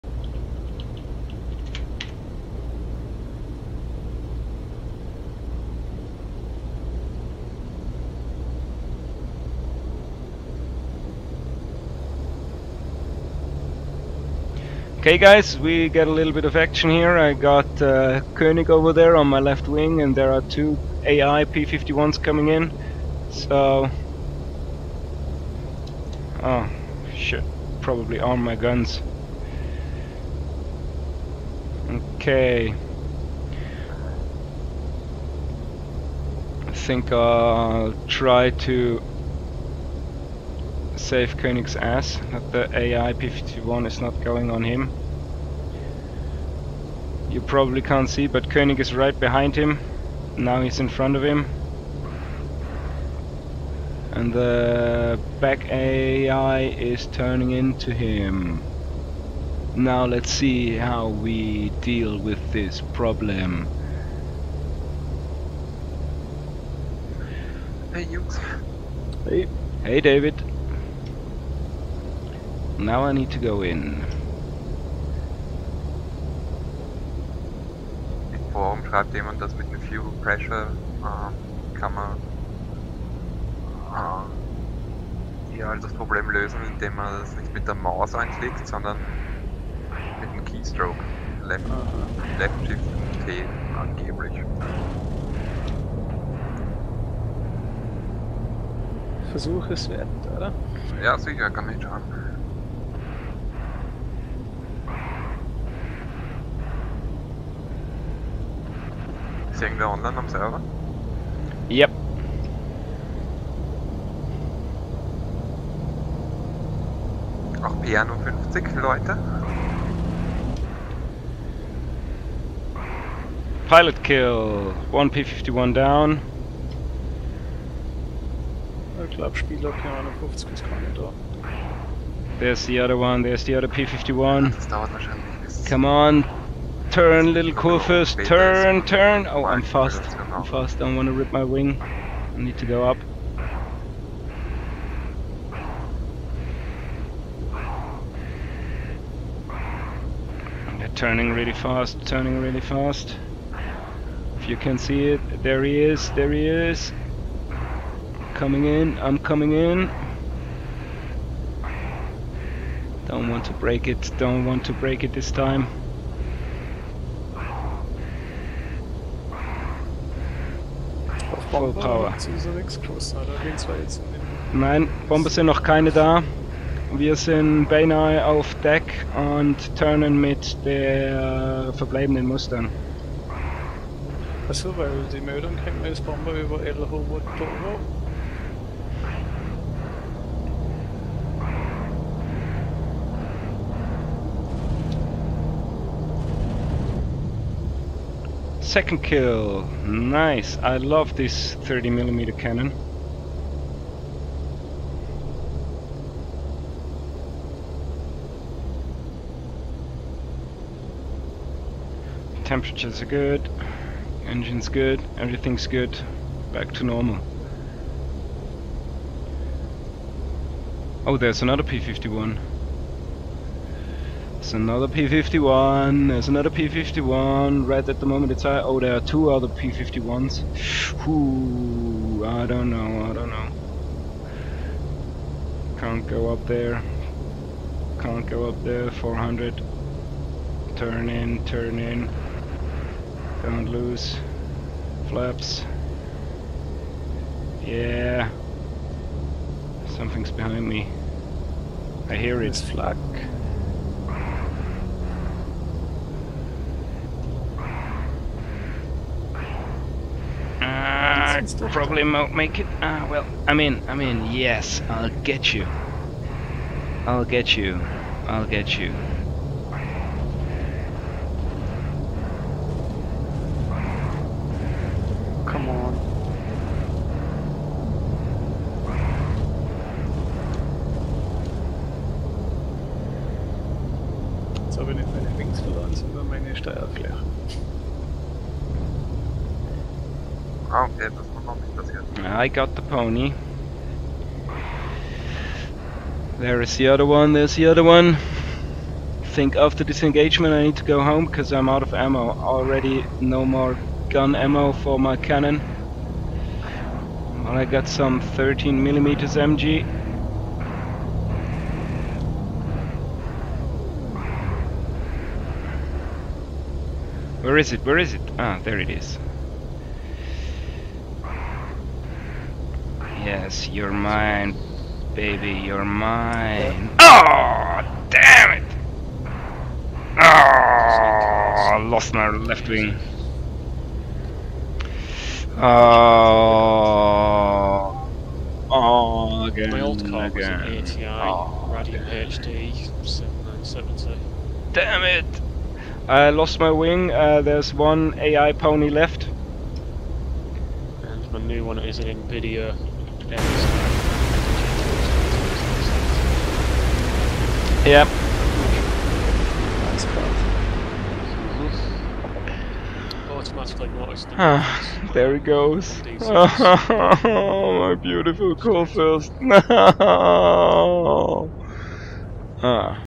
Me-109 K-4 dogfighting on ACG server
see for yourself ^^. with human P51s i probably wouldnt have survived the first encounter ;). but finally the "G" defeated me ^^ -- Watch live